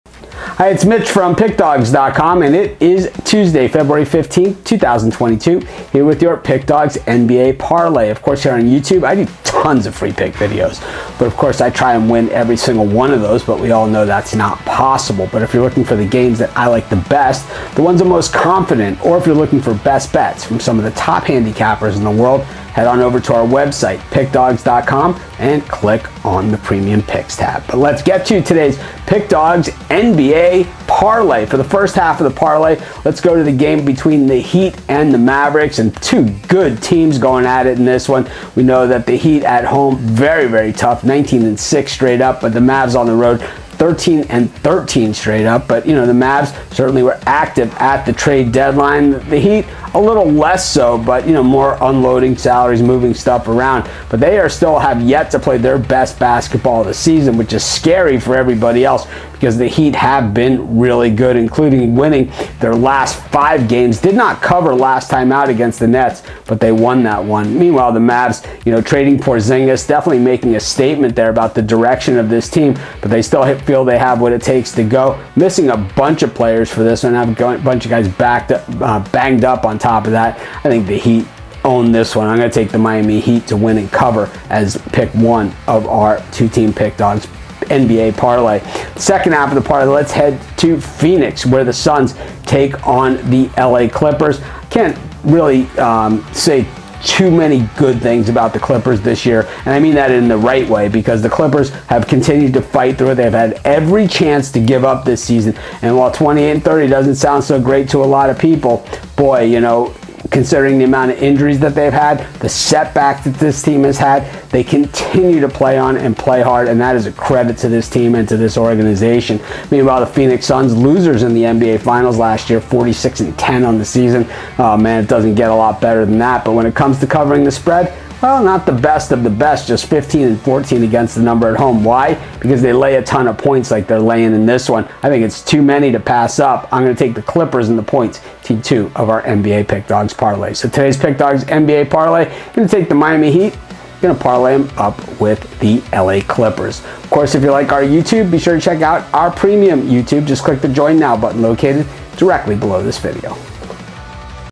Free NBA Parlay For Today 2/15/22 NBA Pick & Prediction NBA Betting Tips
A NBA Parlay for Tuesday.. Get Mitch's NBA Pick and Prediction for this game.